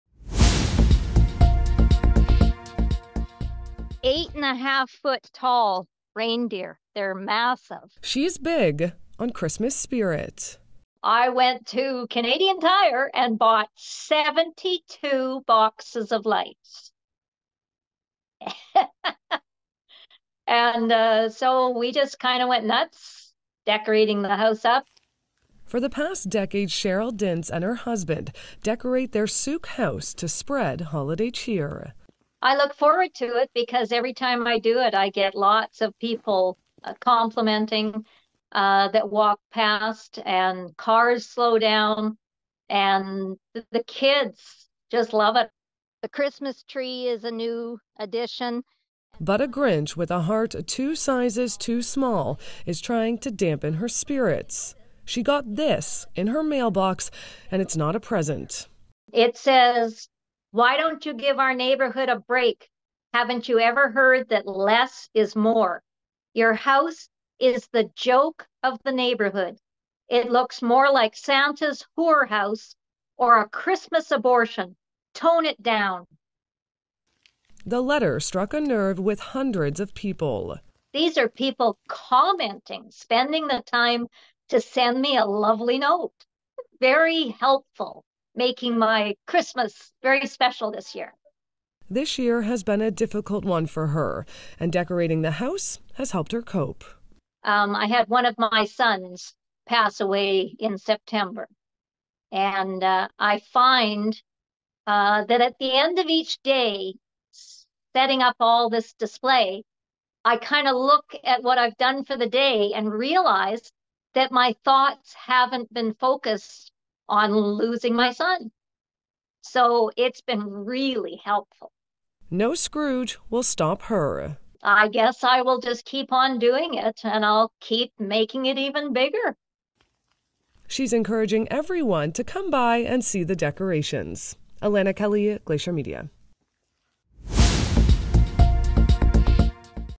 'Tone it down!': B.C. homeowner gets nasty letter over Christmas decorations
The letter used course language, describing the Sooke, B.C., house as 'the joke of the neighbourhood' for using so many Christmas decorations.